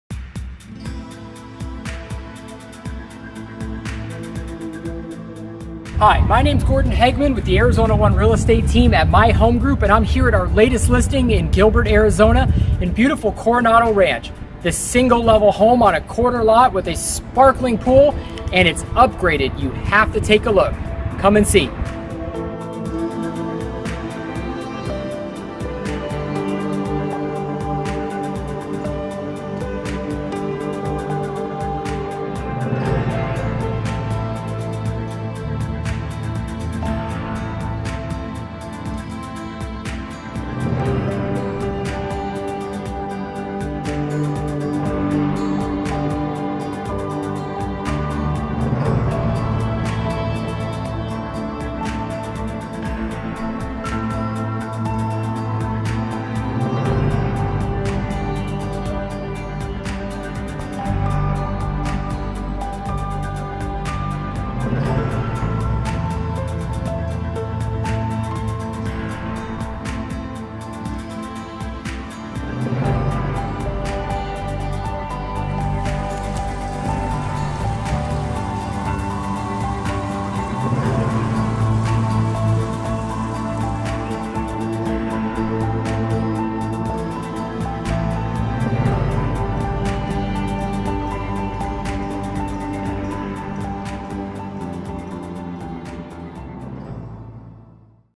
Amazing Gilbert Arizona Home - Coronado Ranch - 3402 E Thornton Ave, Gilbert, AZ 85297
WOW! Don't miss out on the opportunity to sweep up this beautiful home and make it yours! Located in desirable Coronado Ranch on a corner lot just minutes from downtown Queen Creek. As you enter you will feel right at home. Meticulously well-kept home with an HGTV feel. You will love the warm gray toned paint, to the perfectly matched designer wood tile throughout. Gather around the large kitchen island & enjoy the fabulous whitewashed kitchen cabinets, upgraded granite counter tops, subway tile back splash, and newer stainless-steel appliances. There is even a fireplace to enjoy during the cooler months. Head out back to your low maintenance backyard & take a dip in the sparkling pool. Enjoy sitting under the pergola during a warm summer day. Be inspired to make this your forever home!

🆓 FREE RESOURCES:
📝 Preferred Arizona Mortgage Partner: Coming Soon